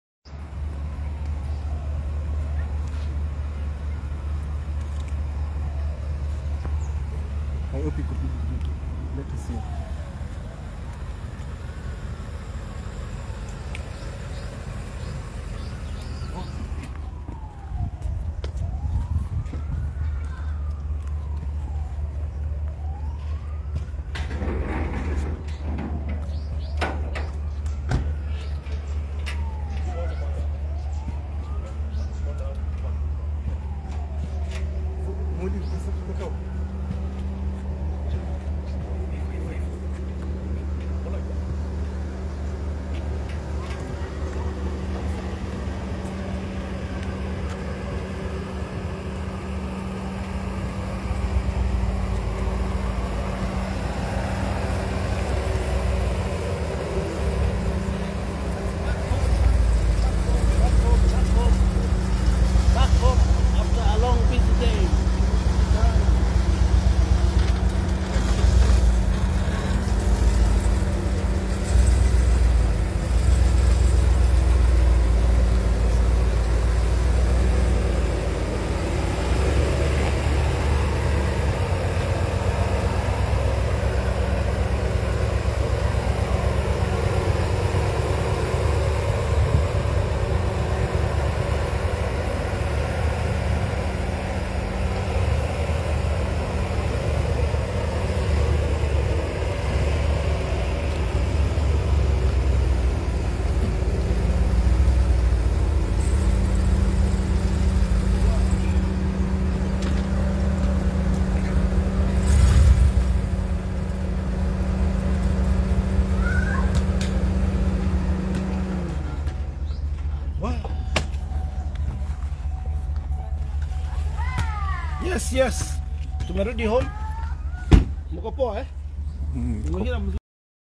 See How I Arrived Home After A Busy Rainy Day In Nairobi
Mike Sonko arriving home after a busy rainy day in Nairobi